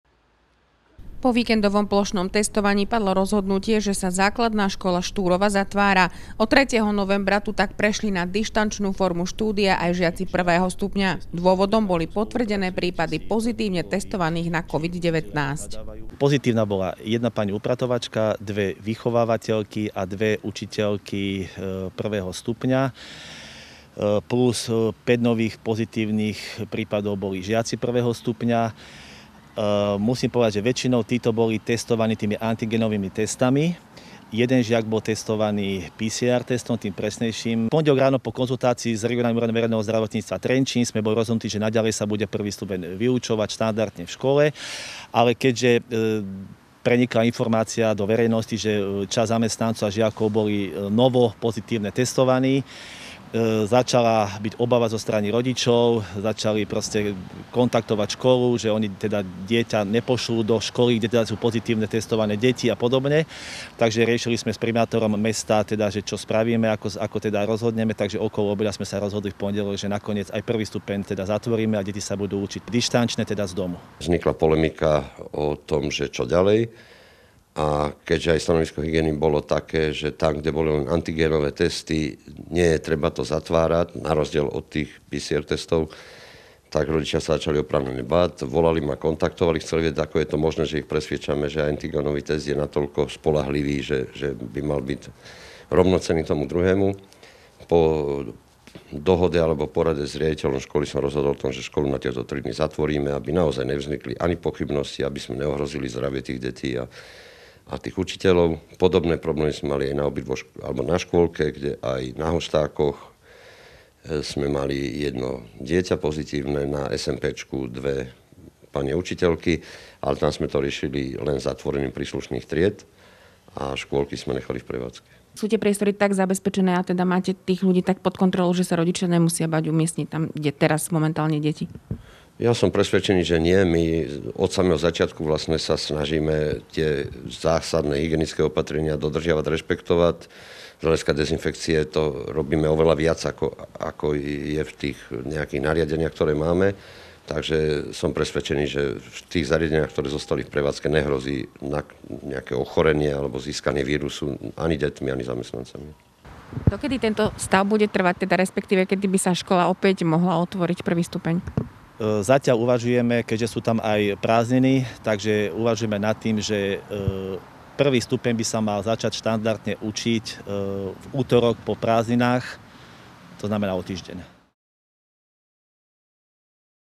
ZŠ Štúrova po celoplošnom testovaní zatvorená
Na škole boli potvrdené prípady na COVID 19